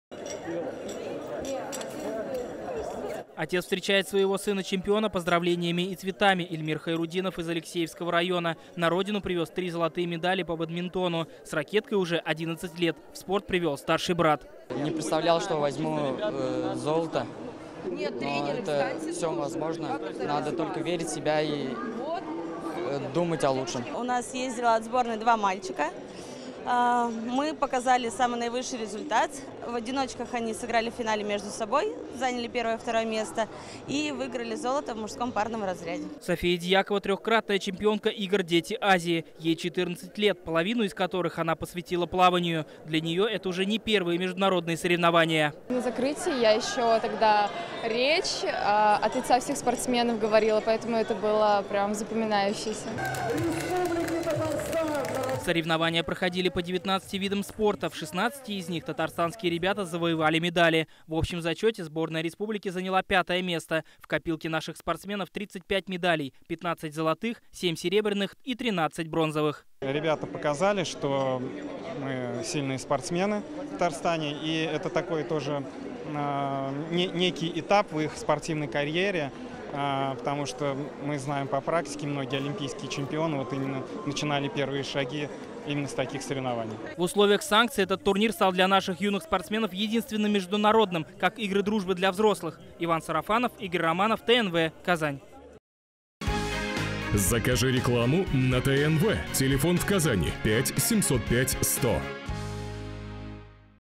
В Казани встретили участников и победителей международных спортивных игр «Дети Азии»
→ В аэропорту Казани встречали 47 спортсменов - участников и победителей международных спортивных игр «Дети Азии». В турнире принимали участие почти 1400 ребят со всех регионов России и 12-ти стран Азии. Они разыграли 199 комплектов наград в 19-ти видах спорта. Это бокс, лёгкая атлетика, плавание, художественная гимнастика и борьба корэш. В общем зачёте сборная Татарстана заняла пятое место.

Координаты:
☎ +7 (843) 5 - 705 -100